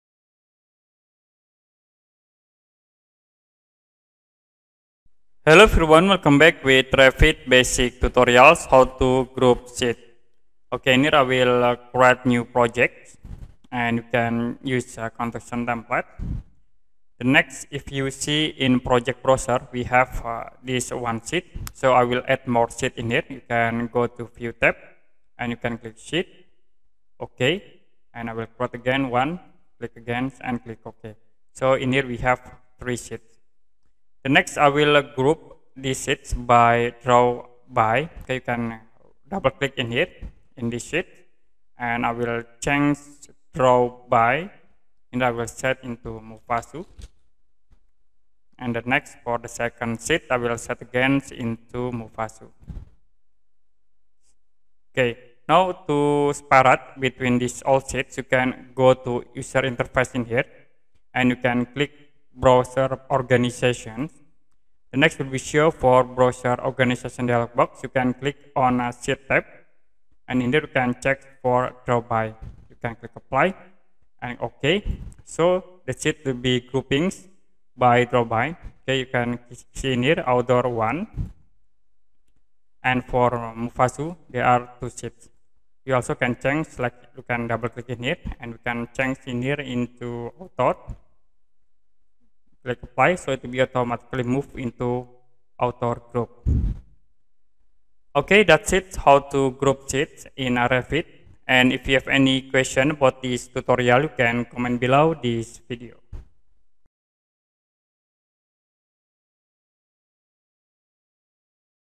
Revit How To Group Sheets Tutorial
Learn autodesk revit basic tutorial for beginner how to group sheet